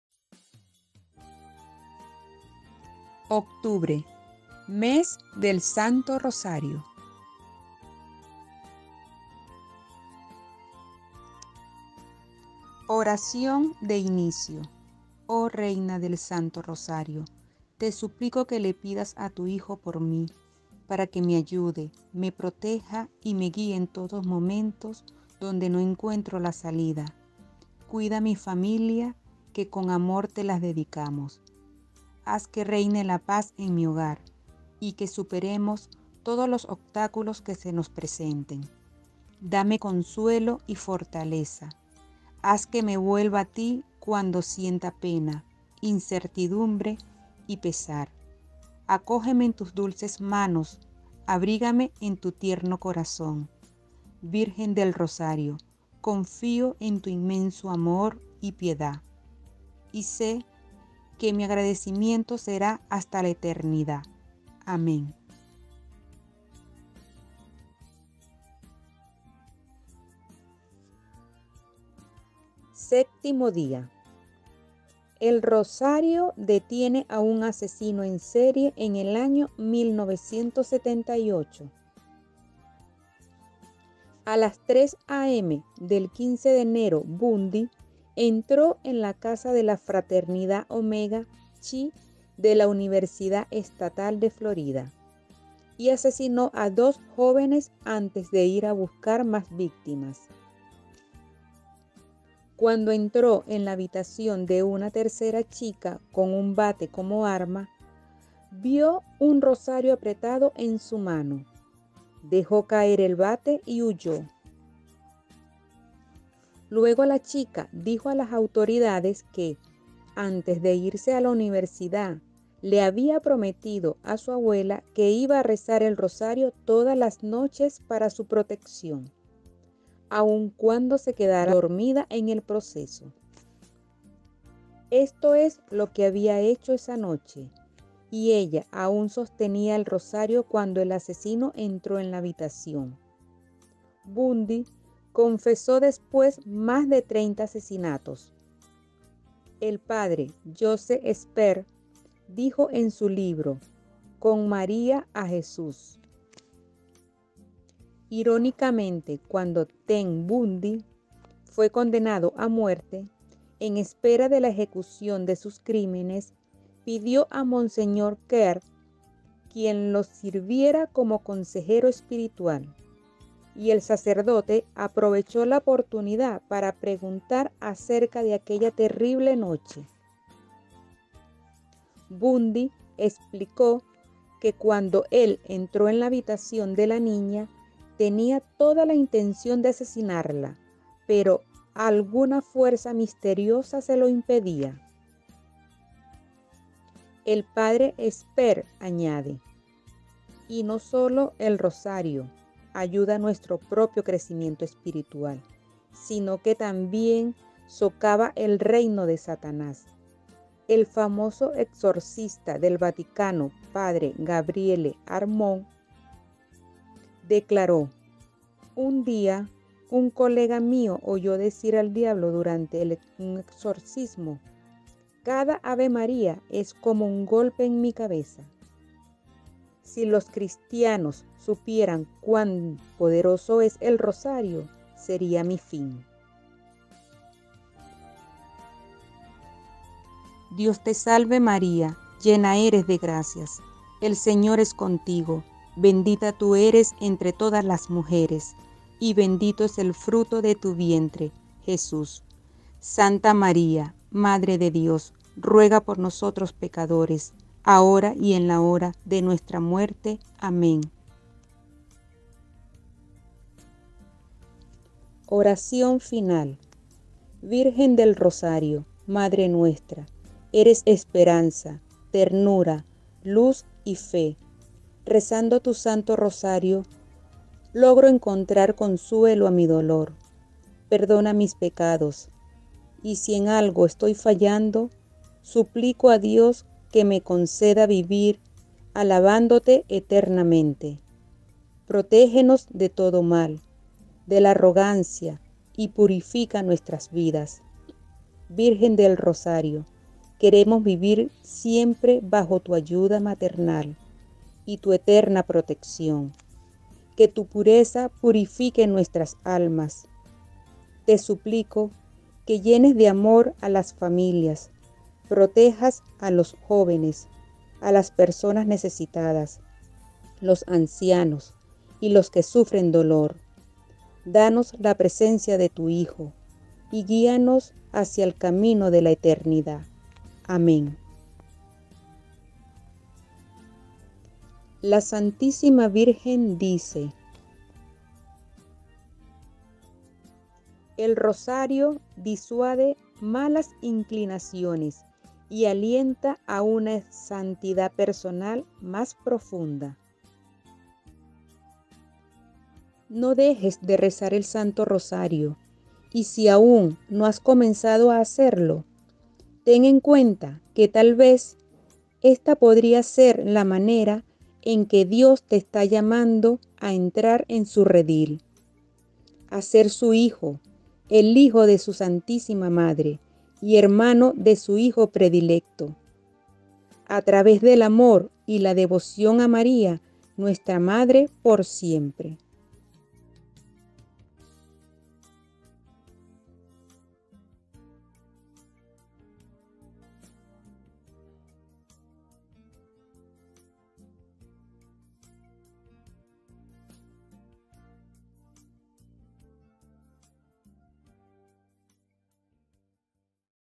EL ROSARIO DETIENE A UN ASESINO EN SERIE. Milagros del Santo Rosario. Día 7
EL ROSARIO DETIENE A UN ASESINO EN SERIE EN 1978

A las 3 a.m. del 15 de enero Bundy entró en la casa de la fraternidad Omega Chi de la Universidad Estatal de Florida y asesinó a dos jóvenes antes de ir a buscar más víctimas. Cuando entró en la habitación de una tercera chica con un bate como arma, vio un rosario apretado en su mano, dejó caer el bate y huyó.
Luego la chica dijo a las autoridades que, antes de irse a la universidad le había prometido a su abuela que iba a rezar el rosario todas las noches para su protección, aun cuando se quedara dormida en el proceso. Esto es lo que había hecho esa noche, y ella aún sostenía el rosario cuando el asesino entró en la habitación. Bundy confesó después más de treinta asesinatos.
El Padre Joseph Esper, dice en su libro “Con María a Jesús”,
“irónicamente, cuando Ted Bundy fue condenado a muerte, en espera de la ejecución de sus crímenes, pidió a Monseñor Kerr quie lo sirviera como consejero espiritual, y el sacerdote aprovechó la oportunidad para preguntar acerca de aquella terrible noche. Bundy explicó que cuando él entró en la habitación de la niña, tenía toda la intención de asesinarla pero alguna fuerza misteriosa se lo impedía“.
El Padre Esper añade:
“Y no sólo (el rosario) ayuda nuestro propio crecimiento espiritual, sino que también socava el reino de Satanás. El famoso exorcista del Vaticano Padre Gabriele Amorth declaró: ‘Un día, un colega mío oyó decir al diablo durante un exorcismo, cada Ave María es como un golpe en mi cabeza. Si los cristianos supieran cuán poderoso es el Rosario, sería mi fin’”.